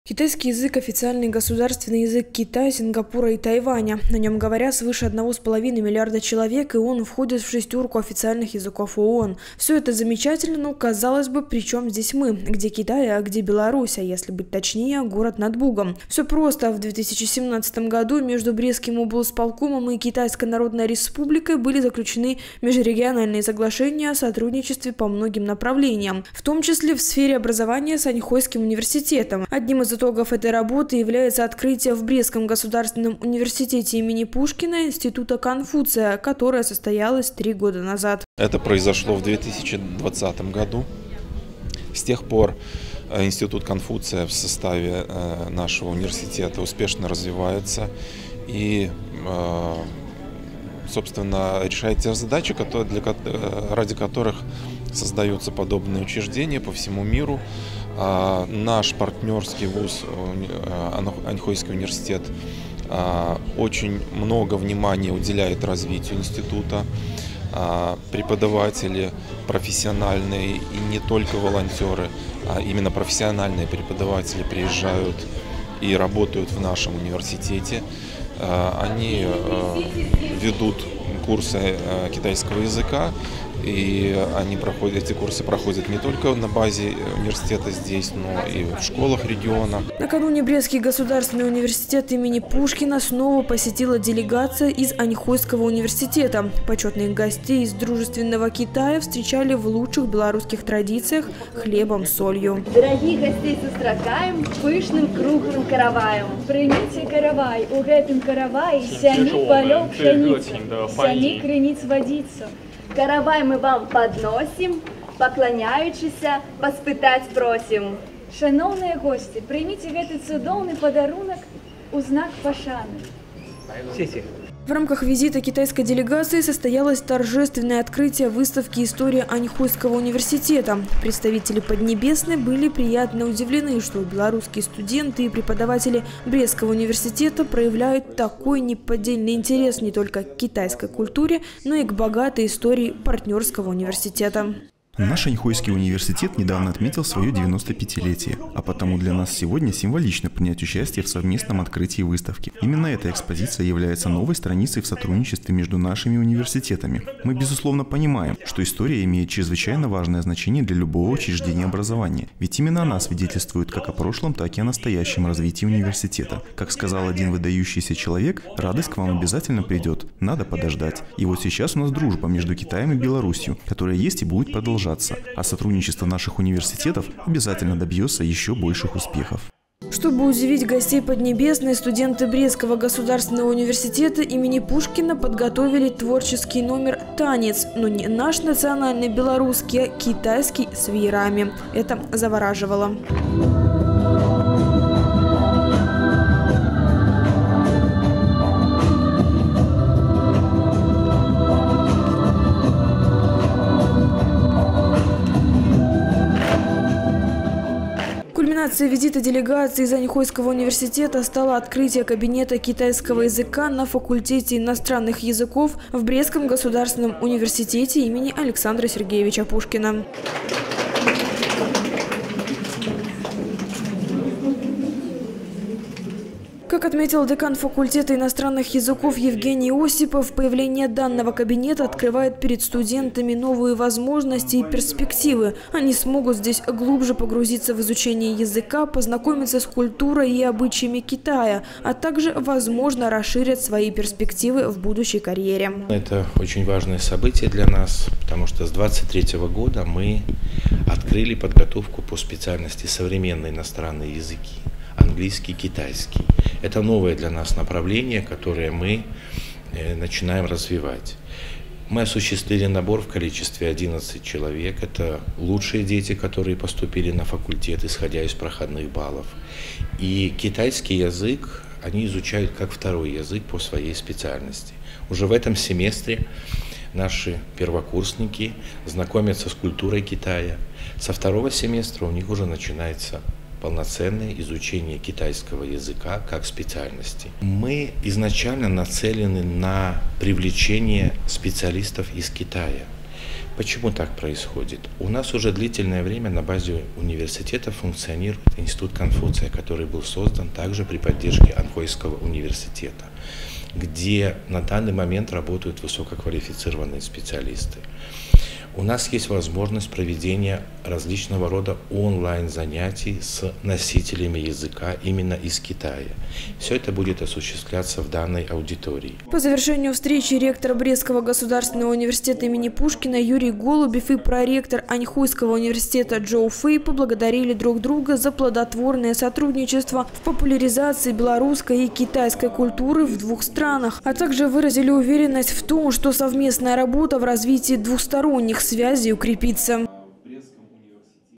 2023-10-26 Визит в БрГУ им. А. Пушкина делегации Аньхойского университета
Изучение китайского языка выходит на новый уровень. И всё это стало возможным благодаря тесному сотрудничеству между ВУЗами двух стран – Брестским государственным университетом имени А.  Пушкина и Аньхойским университетом Китайской народной республики. Совместные проекты объединяют высшие учебные заведения двух стран, в подтверждение тому – работа института Конфуция на базе «пушкинки». А сегодня на базе факультета иностранных языков открыли кабинет китайского языка. Это уникальная возможность для студентов более глубокого изучения языка Поднебесной.  Более подробно в следующем сюжете выпуска.
*Наши контакты:*
✅Телефон горячей линии:
54-63-67